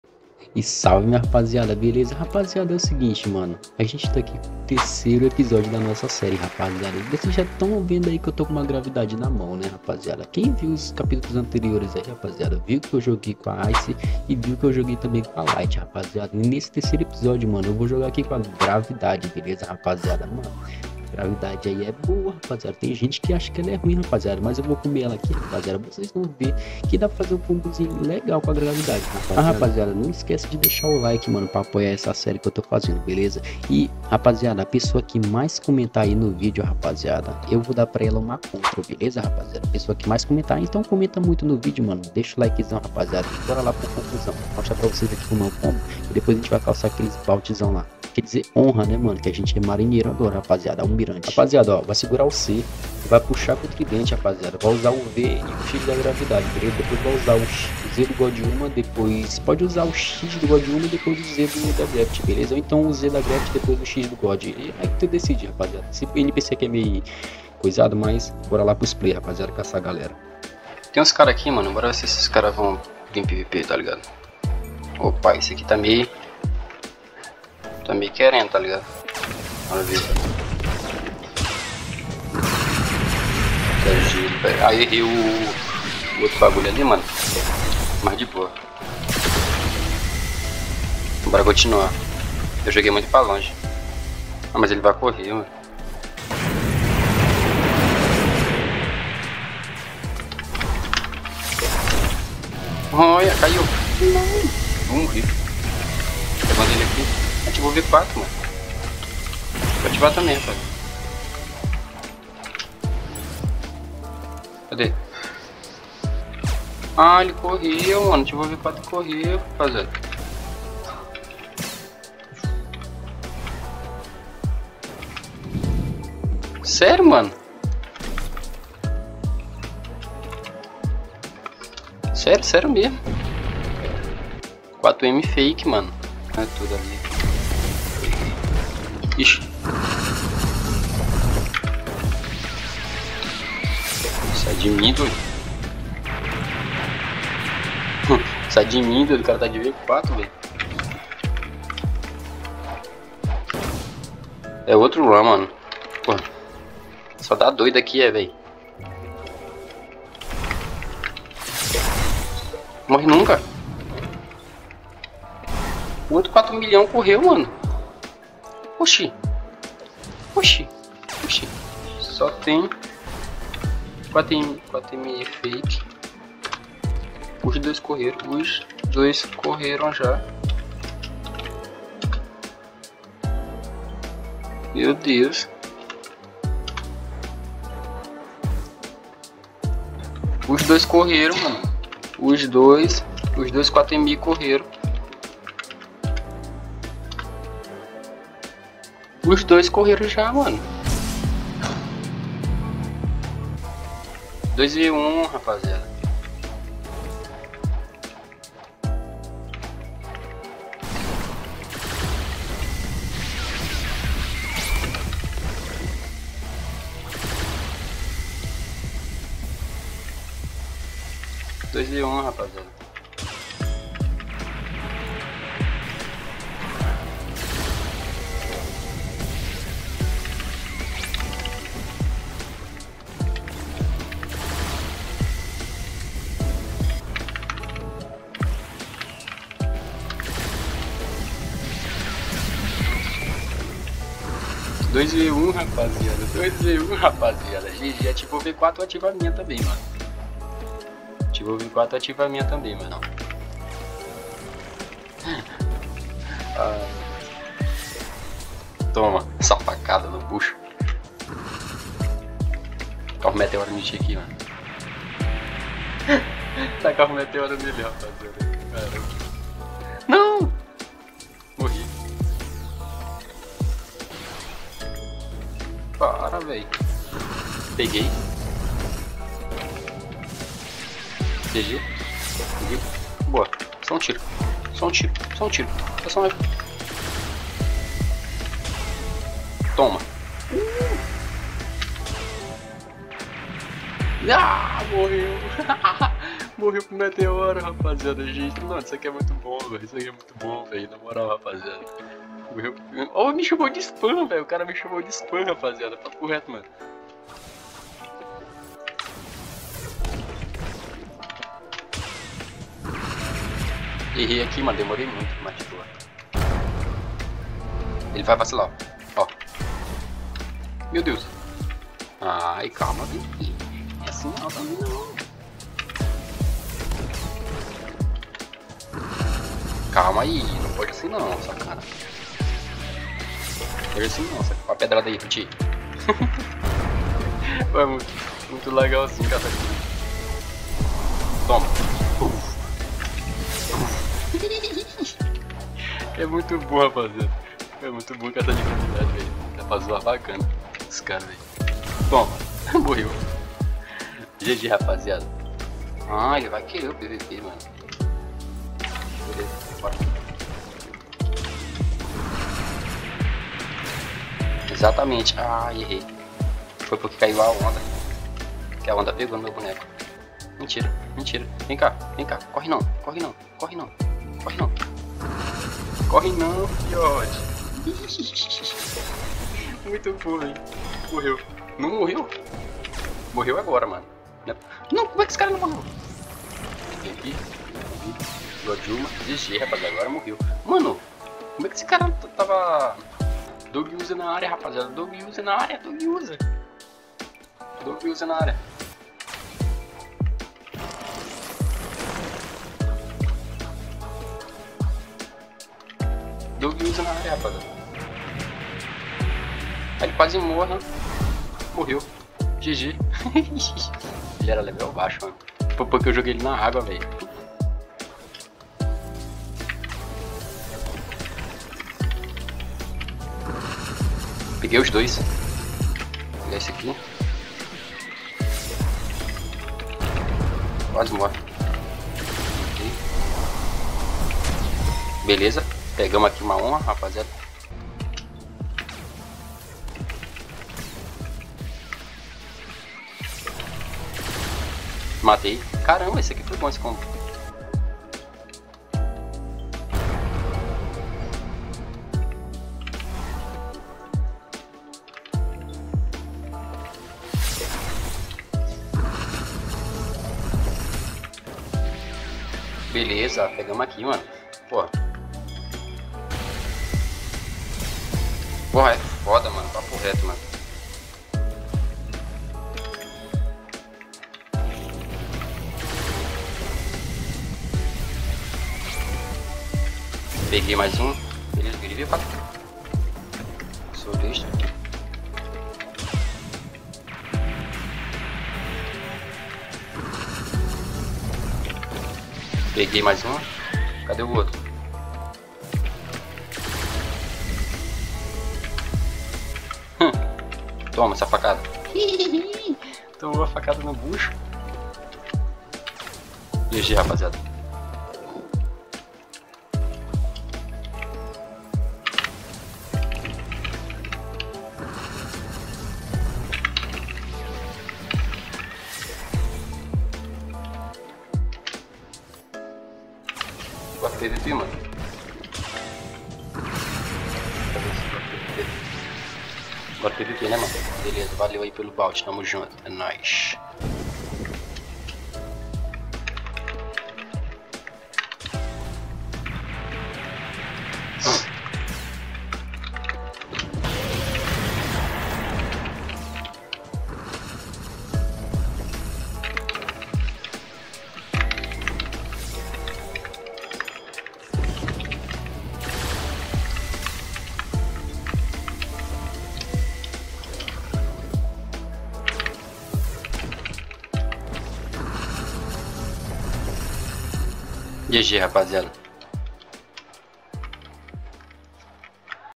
A GRAVITY TA MUITO OP💀 OS PIRATAS TIVERAM QUE CORRER DE MIM😂😱 (Ep.3)
nesse video eu joguei de gravidade e caçei muita honra e os piratas correram pra ela kk